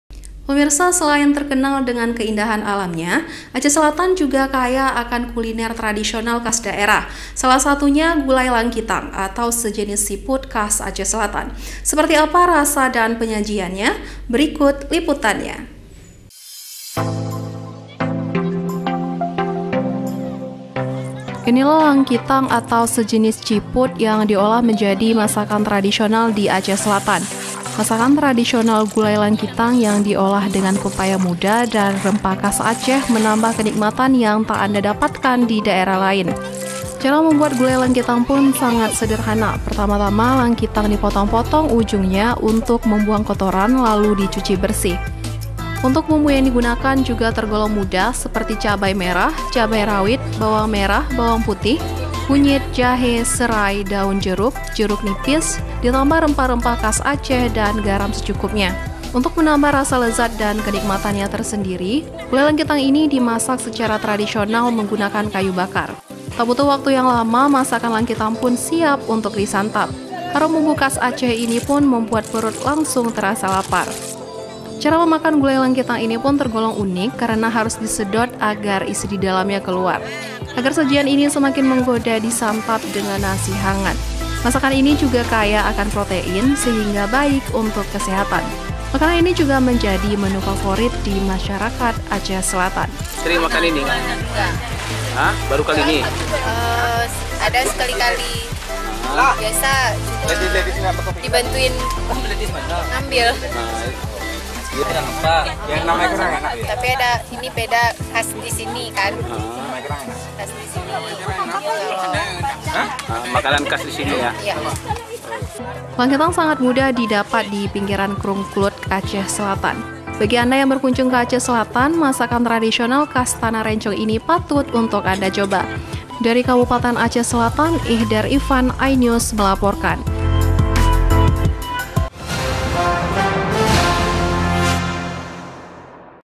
Gulai Langkitang, Sajian Khas Tradisional Aceh Selatan
Kabupaten Aceh Selatan tidak hanya terkenal dengan kekayaan Objek Wisata Alam yang sangat indah. Di Kabupaten tersebut Anda dapat menikmati masakan tradisional Gulai Langkitang atau sejenis Siput Khas Aceh Selatan, yang rasanya  menggugah selera.